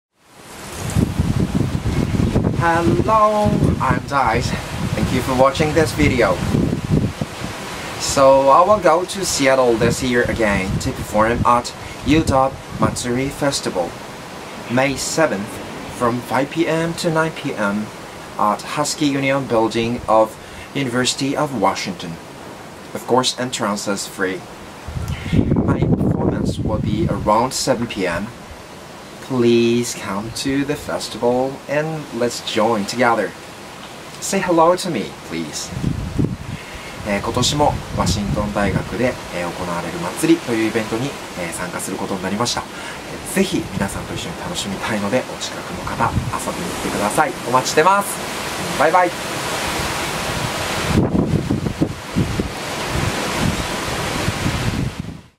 【INFO】d-iZe will go to SEATTLE!! / UW MATSURI 2016
UW Matsuri 2016 and d-iZe LIVE at UW

date / May 7th
time / 5:00pm-9:00pm
place / Husky Union Building (HUB) Ballroom

The annual Matsuri event, hosted by the Japanese Student Association, is a Japanese cultural festival that is filled with traditional games and activities, delicious street-style food, and of course performances!

This year's Matsuri will be held at the Husky Union Building at the University of Washington on May 7th from 5pm to 9pm.

Admission is free but tickets for games and food will be available at the HUB front desk and JSA tabling booth.